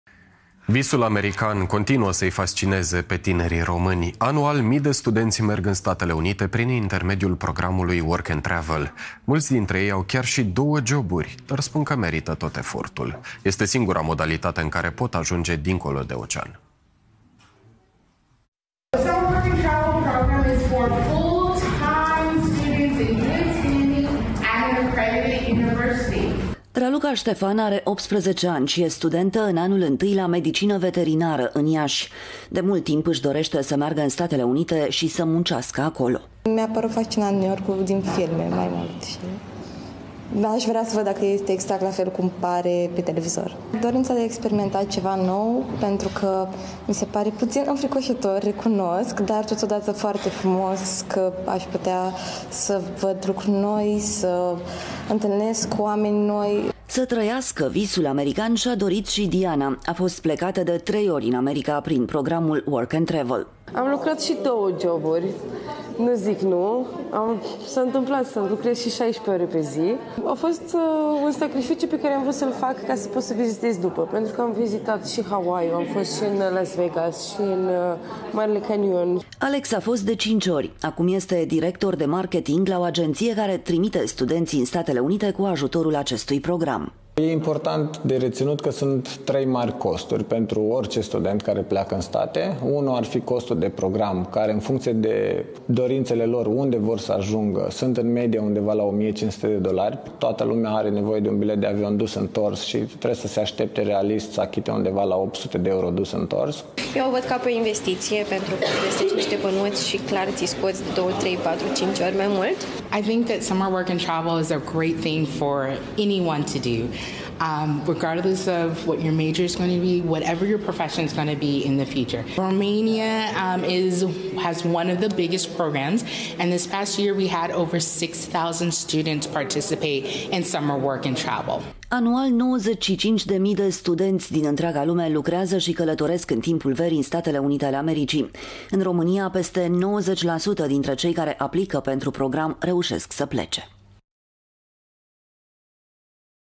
”Work and travel”, o altfel de vacanță de vară. Anual, 7 mii de studenți din România ajung în SUA
Visul american continuă să-i fascineze pe tinerii români. Anual, mii de studenţi merg În Statele Unite prin intermediul programului Work and Travel.

Mulți dintre ei au chiar și două joburi dar spun că merita tot efortul. Este singura modalitate în care pt ajunge dincolo de ocean.

Anual, aprope 7 mii dintre cei 95 de mii de studenți care pleacă în străinătate sunt din România.